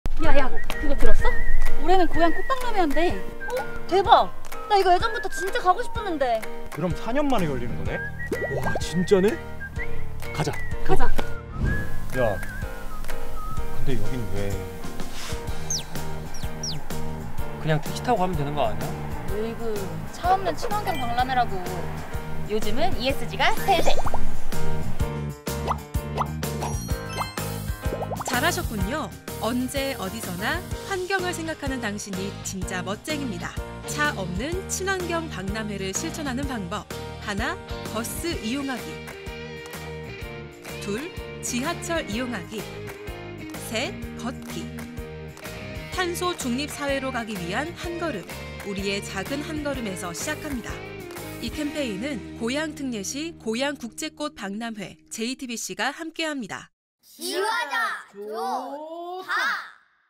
지구를 위한 작은 실천, 탄소중립 친환경 박람회 2023고양국제꽃박람회
2023고양국제꽃박람회는
고양특례시, JTBC와 함께
차없는박람회 공동 캠페인을 펼칩니다.

ESG 경영을 실천하는 고양국제꽃박람회는
탄소중립 친환경 박람회로 앞장섭니다!
지구를 위한 작은 실천, '고양꽃박'과 함께해주세요~